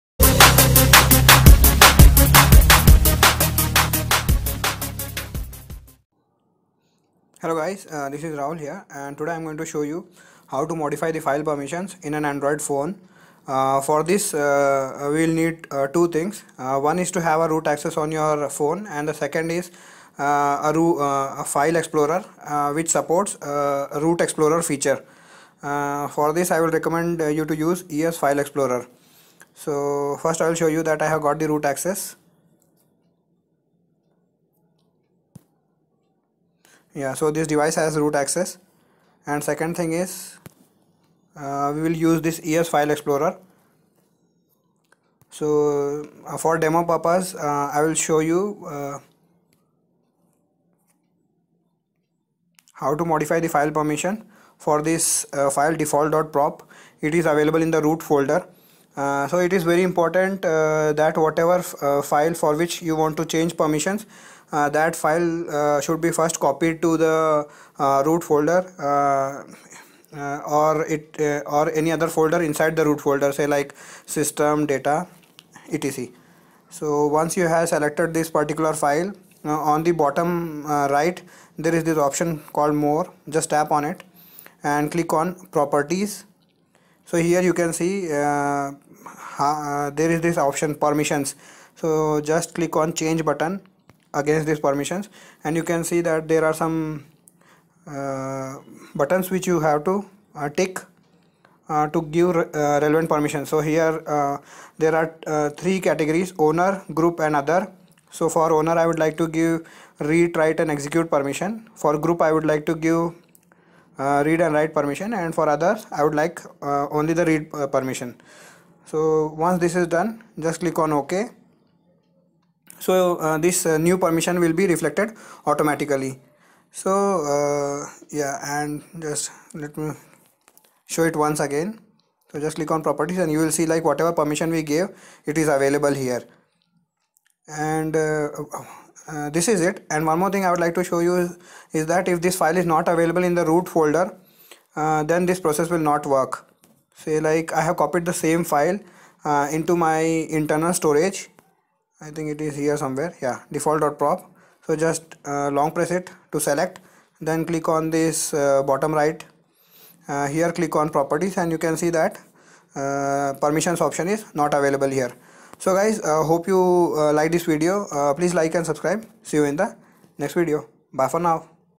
Modify File Permissions On Android via ES File Explorer
Hello everyone!!

In this video, i show how to modify file/app permissions on android phone using ES File Explorer.

Cheers!!